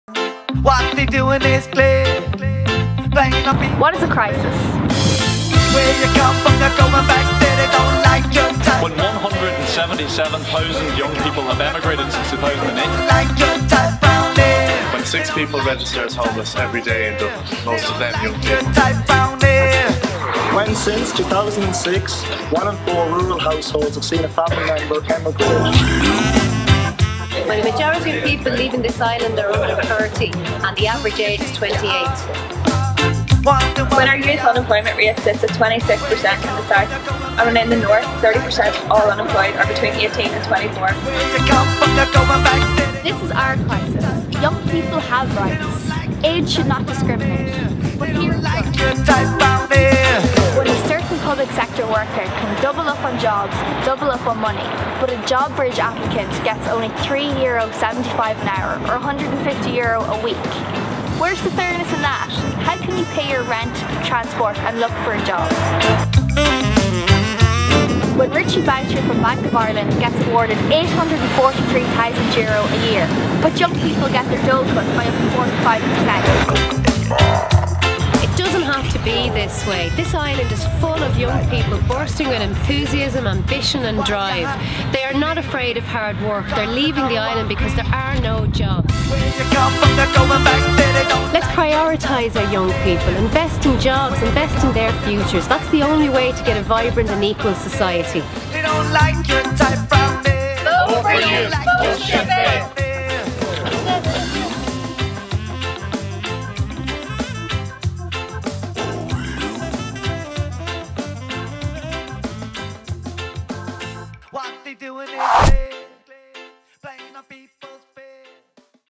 Youth matters - Not for export
Sinn Féin Youth Affairs Spokesperson, Senator Kathryn Reilly, and Dublin EU candidate Lynn Boylan launched the party's proposal document to tackle issues affecting young people in Ireland.

Following on from months of consultation with youth groups and organisations across the island, the document, Youth Matters: Not For Export!, identifies the biggest issues facing young people today, the government's response to date and deficiencies in the Youth Guarantee.

(Video by: Michael Nolan, Mark Moloney and Kaniah Cusack)